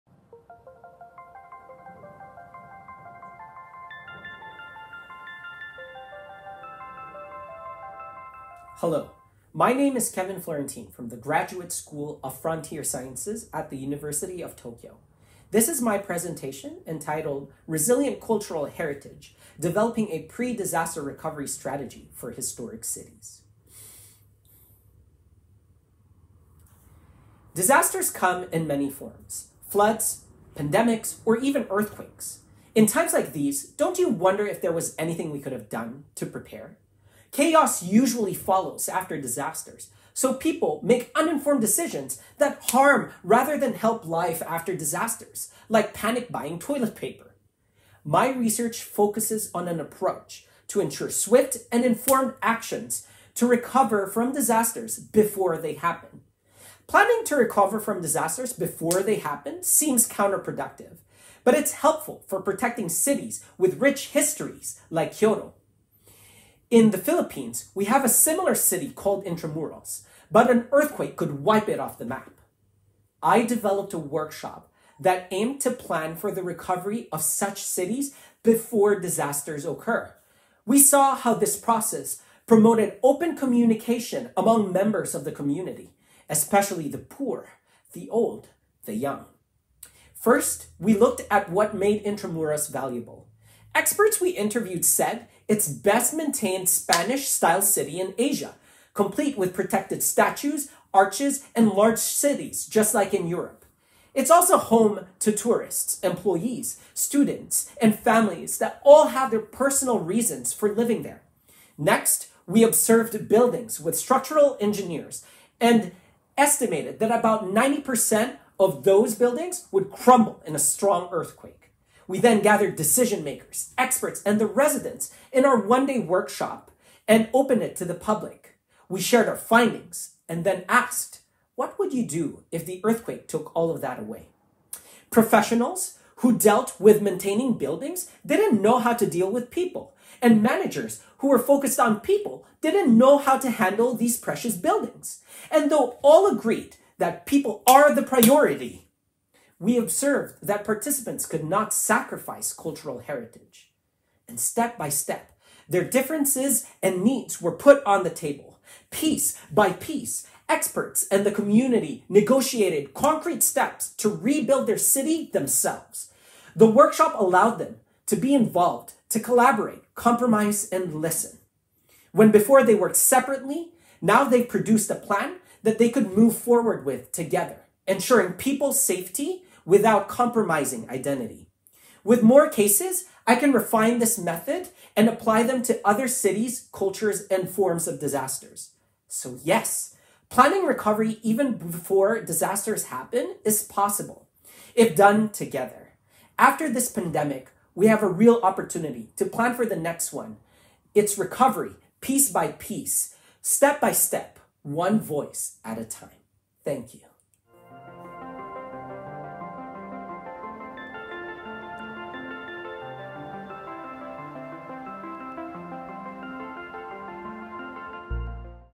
Three Minute Thesis (3MT) 2020 - Kevin Florentin
Kevin Florentin - "Resilient Cultural Heritage: Developing a Pre-Disaster Recovery Strategy for Historic Districts"

The University of Tokyo held its second Three Minute Thesis competition on Saturday, May 23, 2020 as a fully virtual event.  Nineteen Ph.D. students introduced their research in just three minutes using a single, static slide. The students independently recorded their presentations at home. Prizes included a Third Place Award, a Runner-Up Award, and a Grand Prize Winner (who will represent UTokyo at the 2020 Asia-Pacific 3MT).

3MT® is a registered trademark of The University of Queensland.